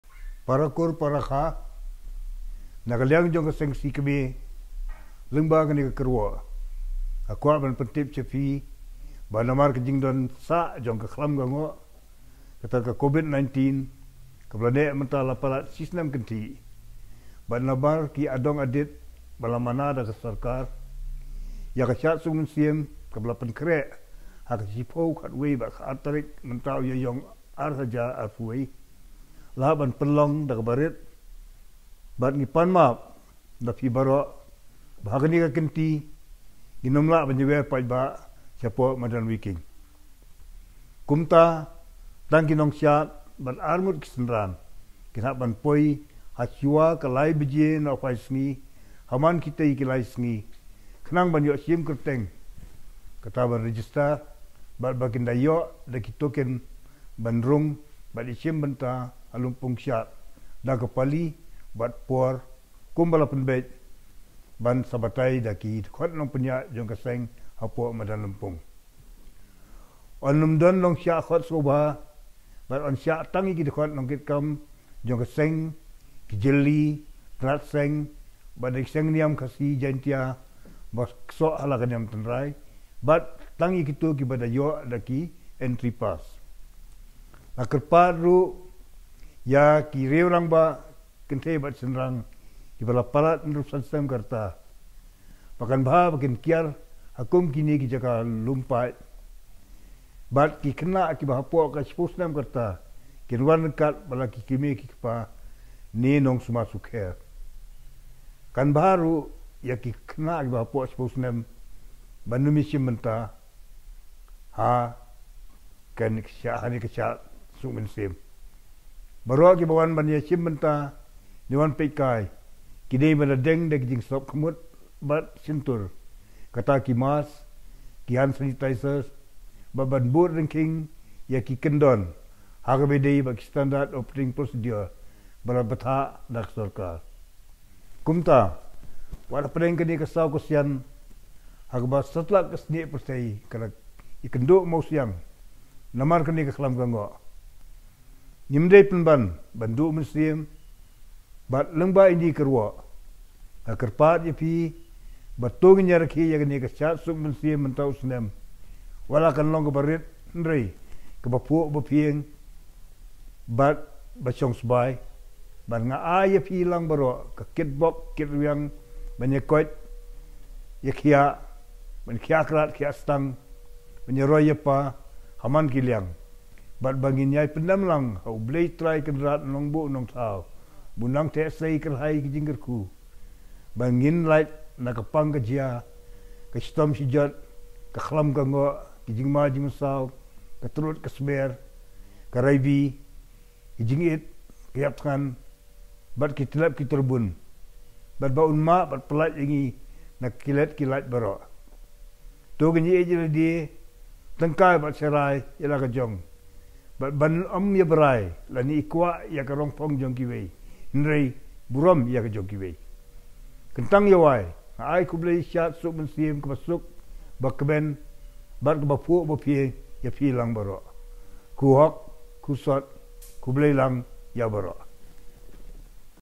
Shad Suk Mynsiem 2021 | Kyrwoh | Seng Khasi Kmie
Ka Kyrwoh Shad Suk Mynsiem 2021 na u President Seng Khasi Kmie, Rgh DK Dkhar